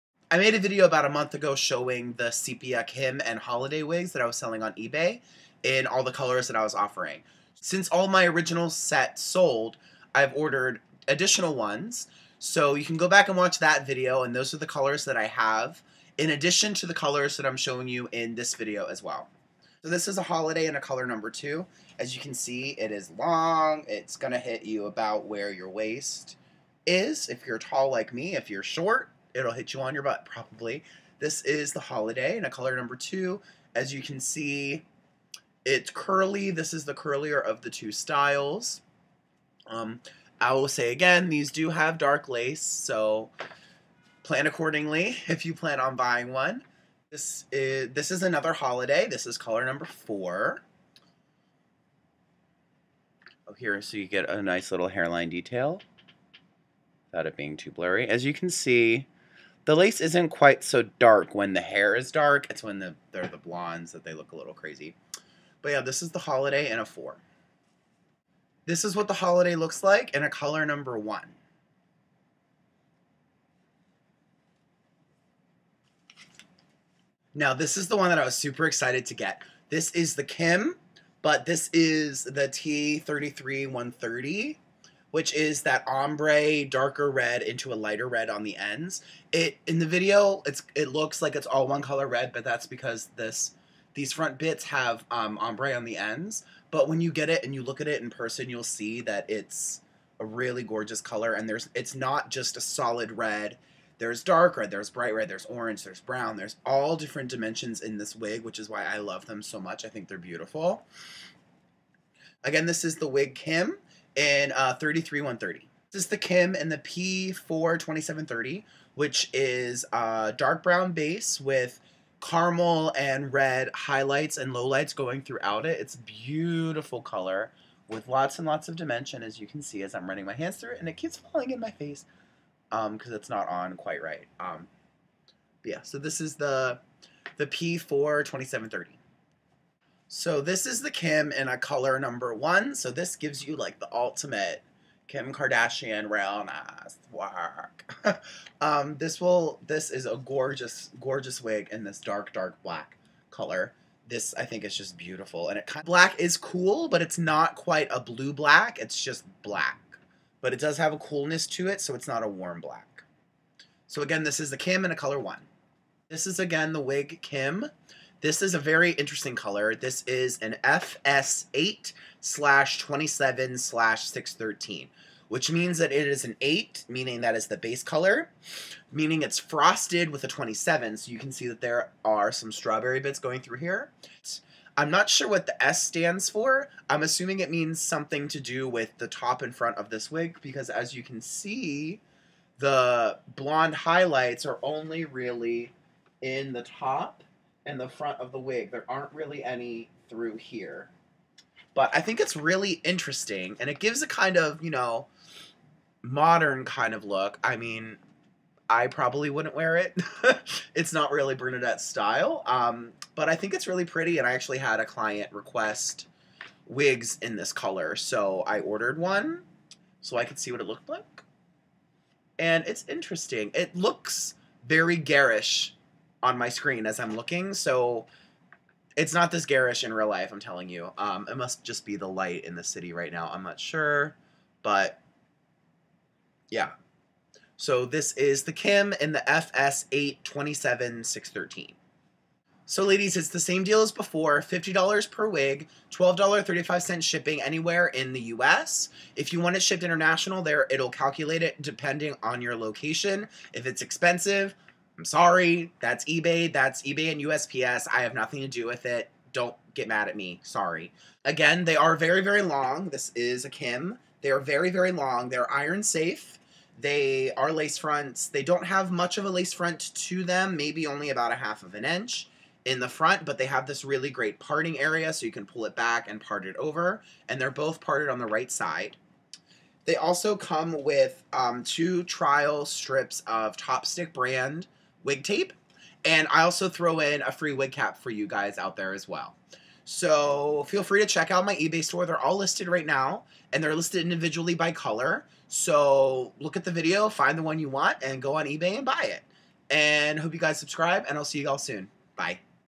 REVIEW - Sepia "Holiday" & "Kim" Lace Front Wigs Part 2
Hey Wiglets!!!

Today I am doing a review and showcase of a few new Lace Front Wigs I have in stock and available on eBay!

These are gorgeous heat friendly synthetic Lace Front wigs by Sepia brand. They are styles "Holiday" the curlier version and "Kim" the wavier version.

 * MY FUN SOCIAL MEDIA LINKS